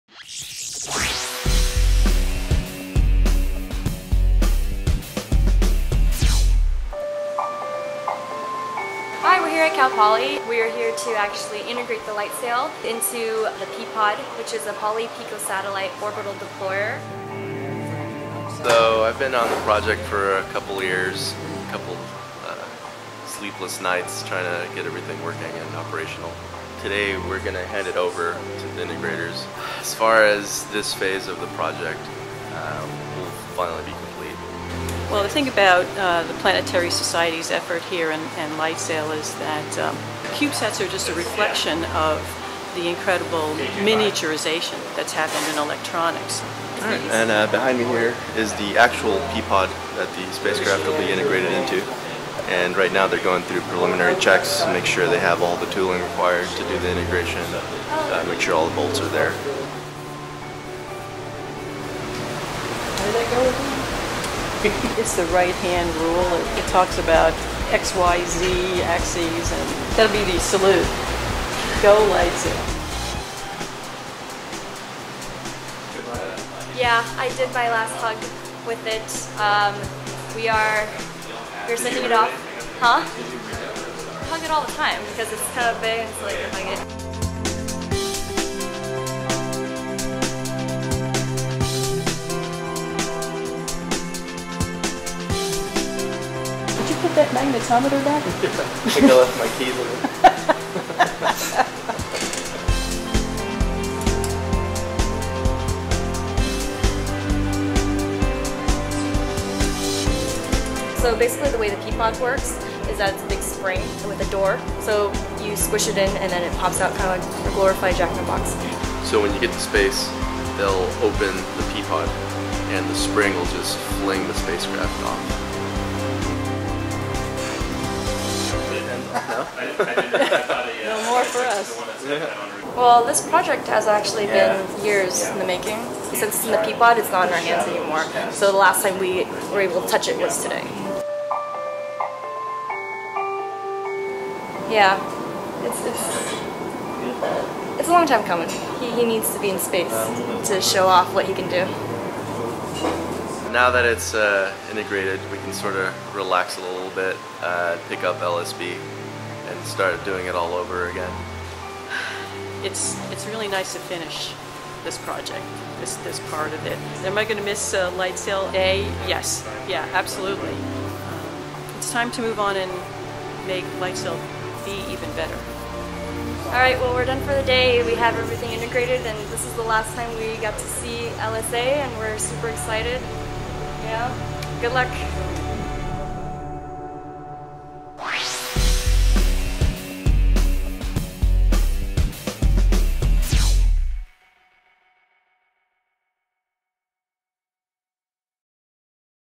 LightSail Integration - SciTech
In January 2015, the LightSail team had their last chance to touch the LightSail-A Spacecraft. They handed it over to Cal Poly where it was integrated into the P-POD (Poly-Picosatellite Orbital Deployer) for its May 2015 launch. Go LightSail!